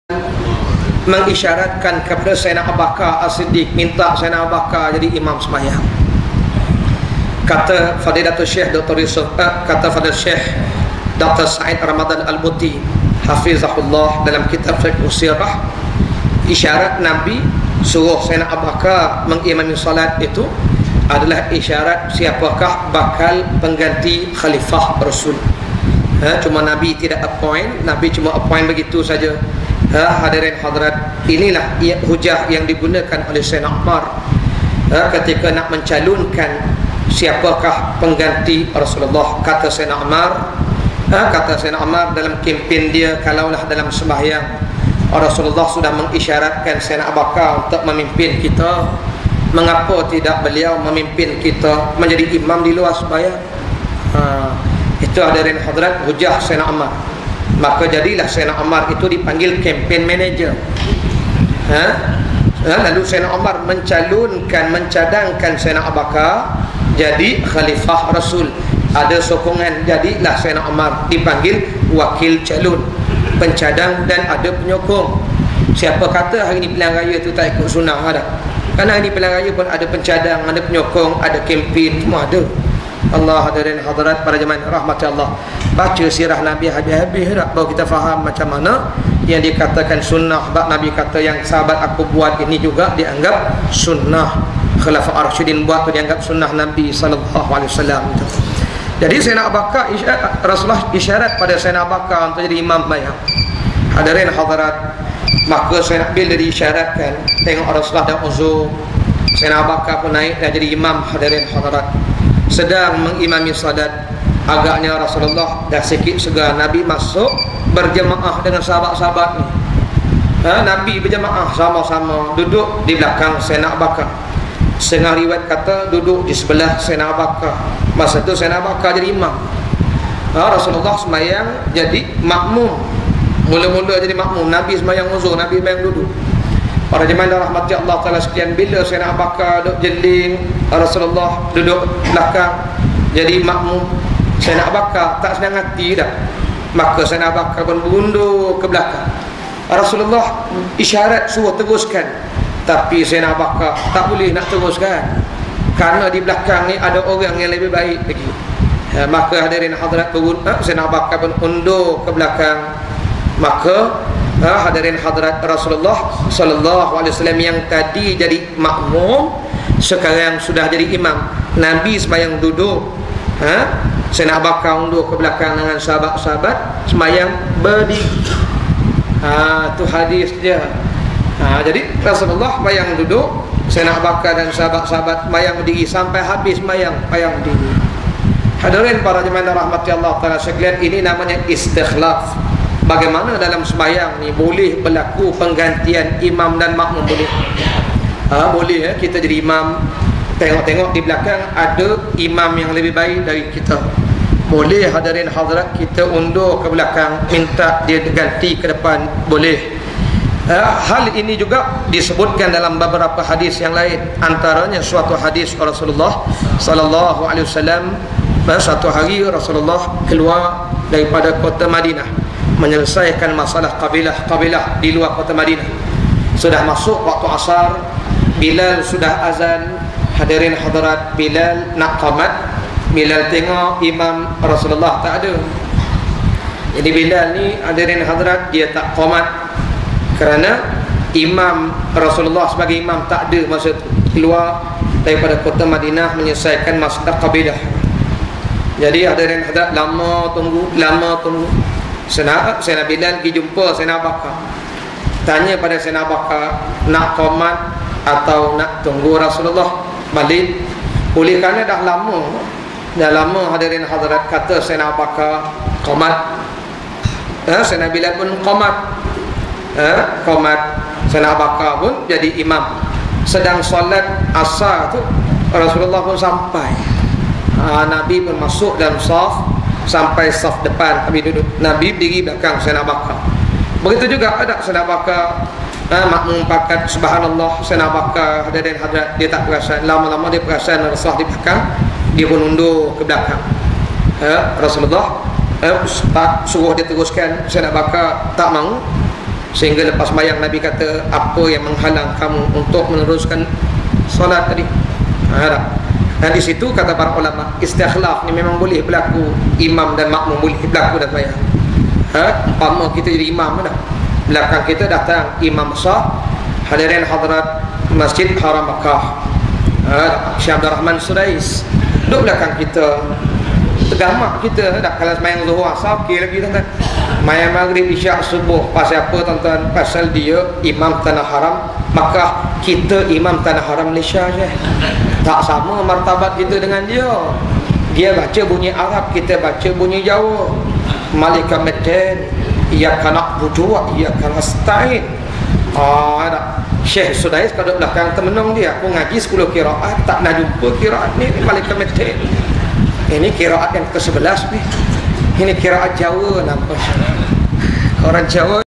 Ustaz Raihan - Solat Duduk Berjemaah (Bhg 6)
Ustaz Raihan Abdul Aziz (Bicara Hadith)
Surau At Taqwa
Seksyen 7, Shah Alam
1 Mei 2012